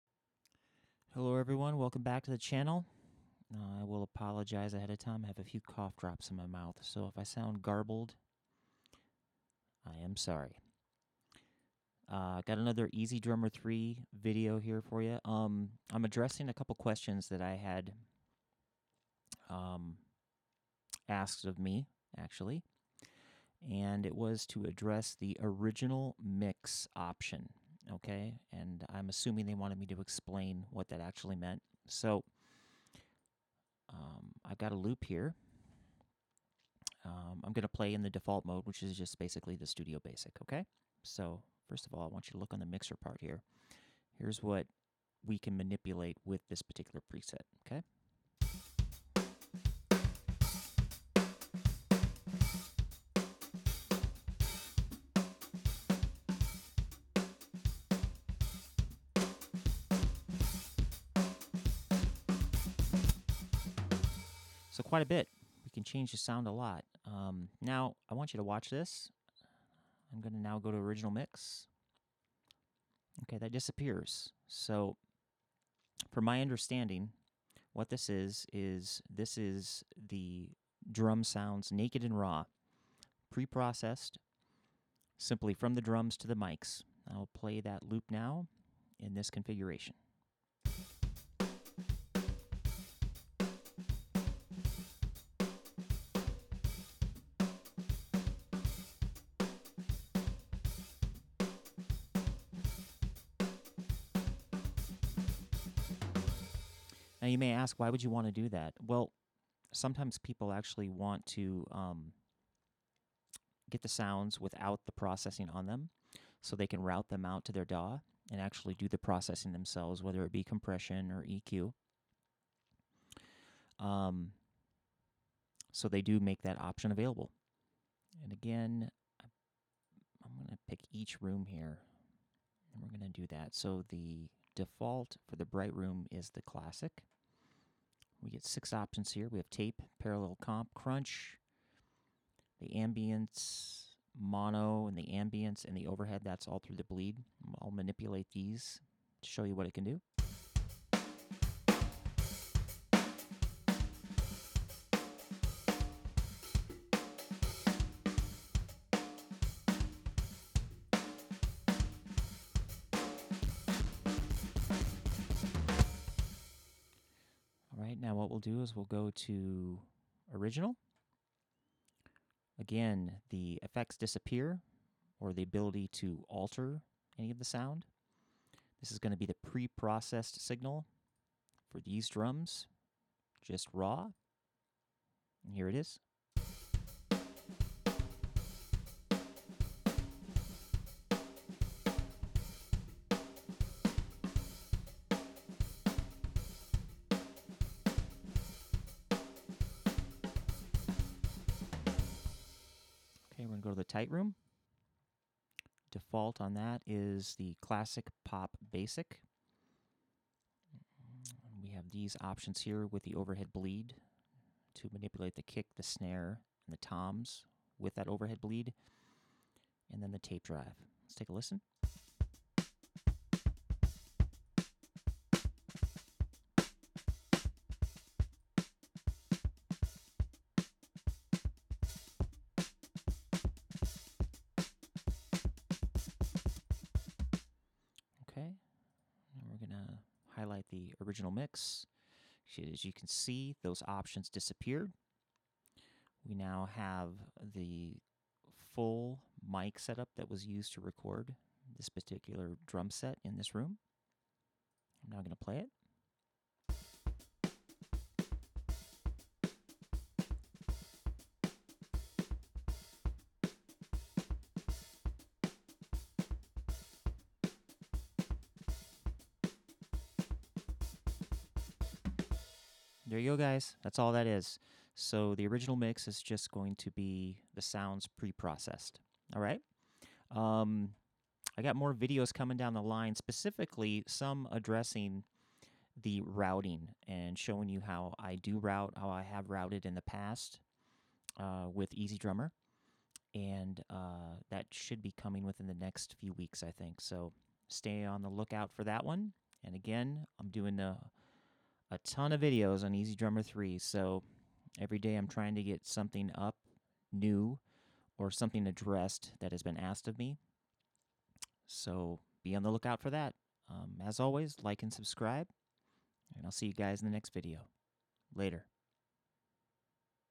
Explaining the "Original Mix" option in EZDrummer3
Tutorial on the difference between a preset and the original mix option in regards to the mixer window esp.